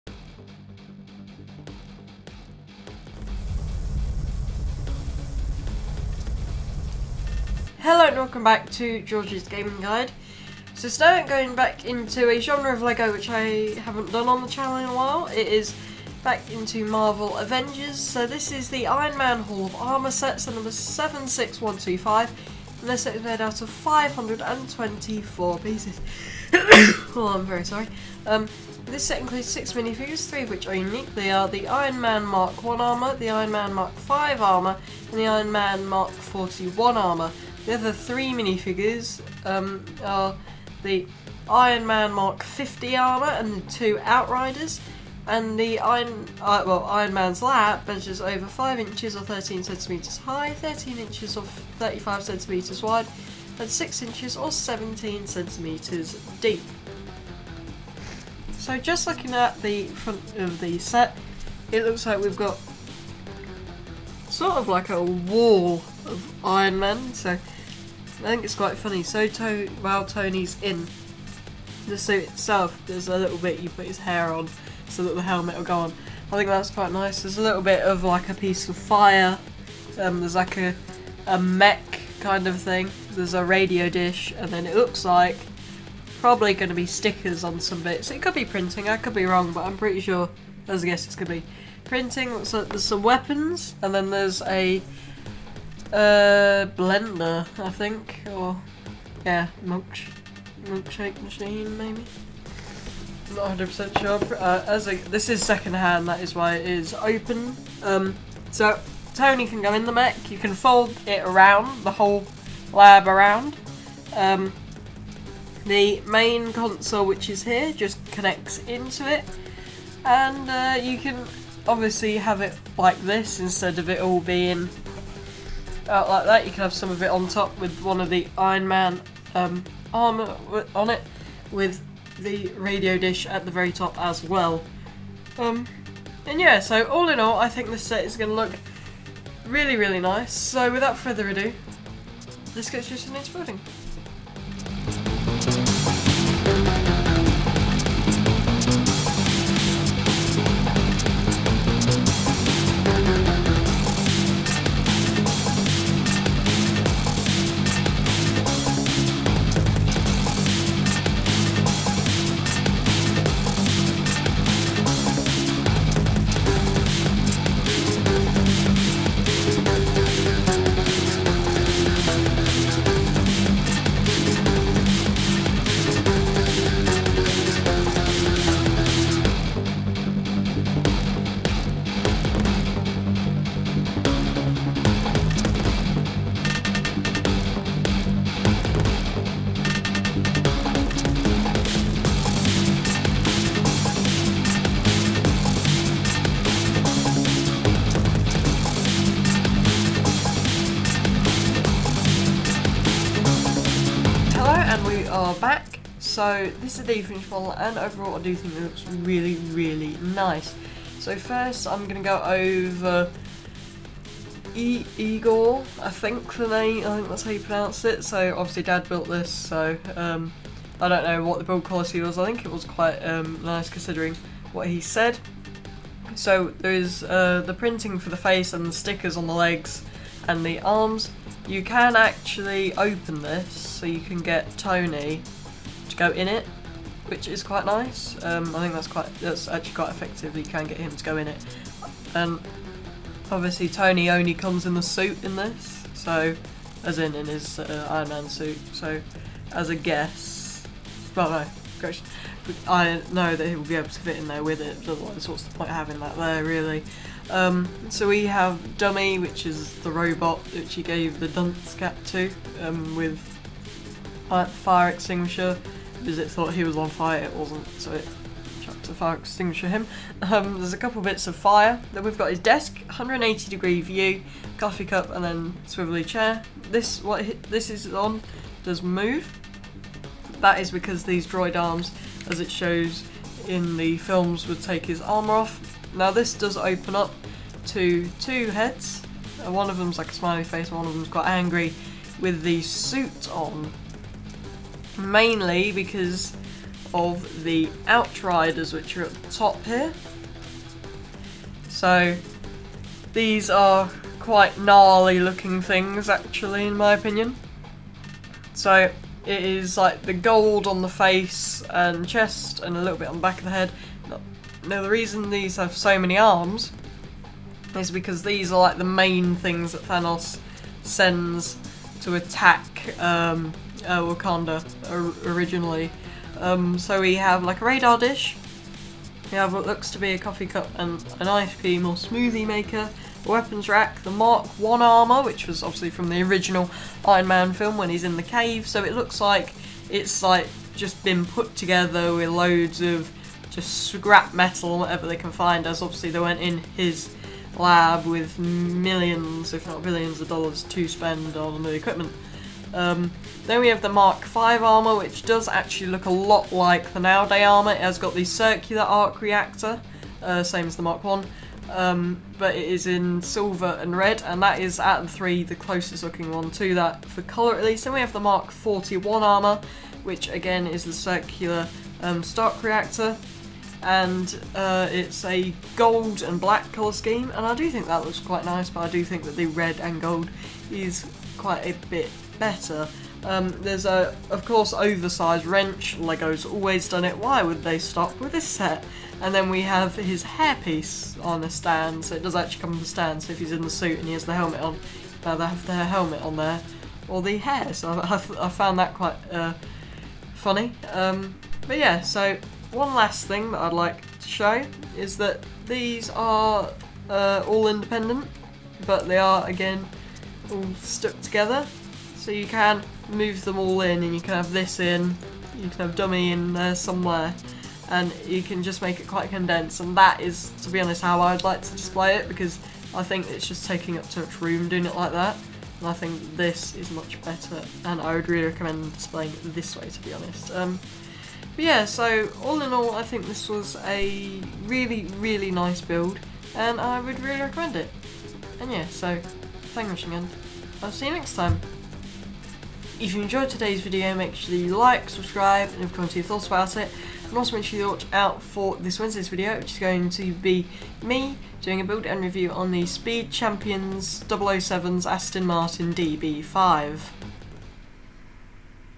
LEGO Marvel Iron Man Hall of Armour (76125) Build and Review
Today we have LEGO Marvel Iron Man Hall of Armour, this is set number 76125, comes with 524 pieces and was released in 2019.

Conduct experiments with Tony Stark and capture the Outriders with the LEGO Marvel Avengers 76125 Iron Man Hall of Armor, featuring a modular lab that can be combined and stacked in many different ways for endless play possibilities. The hall includes a rotating podium with 2 posable robotic arms for Tony Stark to get suited up as Iron Man, a desk, kitchen, weapon storage, tool storage and Iron Man suit storage modules, a radar dish and more. This versatile Iron Man construction toy includes a posable Igor Suit mech with minifigure cockpit, a posable mobile robot and 6 minifigures—including Iron Man MK 1, Iron Man MK 5, Iron Man MK 41 and Iron Man MK 50, plus Tony Stark face and hair elements to attach to your chosen Iron Man minifigure. This set also includes 2 Outriders.

The Iron Man lab measures over 5in. (13cm) high, 31in. (35 cm) wide and 6 in. (17 cm) deep and will look great in any one's collection.

Chapters:-
00:00 Logo
00:07 Intro
02:23 Build
03:23Review
07:22 Coming up